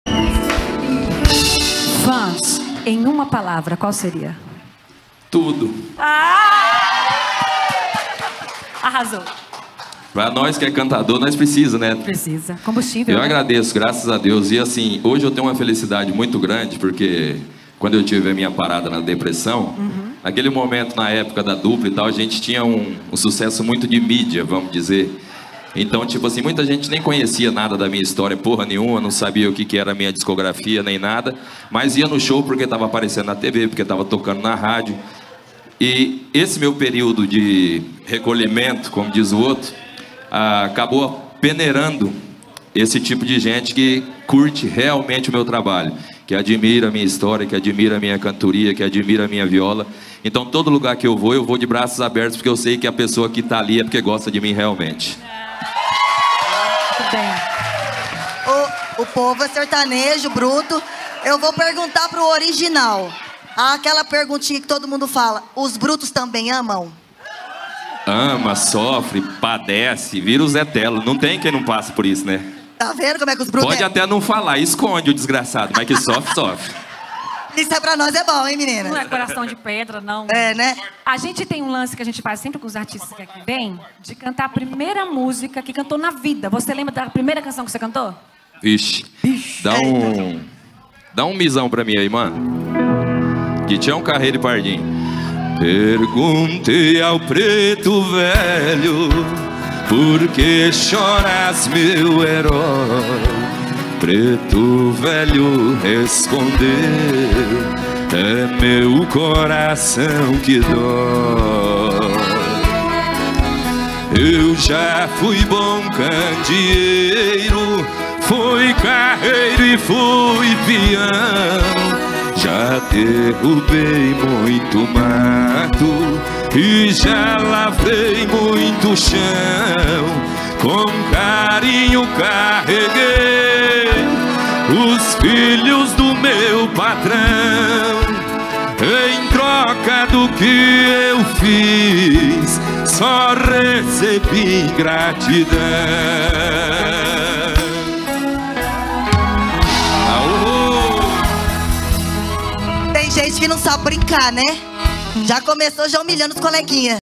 João Carreiro respondendo de forma carinhosa pergunta sobre os fãs.
João Carreiro aqui demonstrando como sempre o seu carinho com o seu público.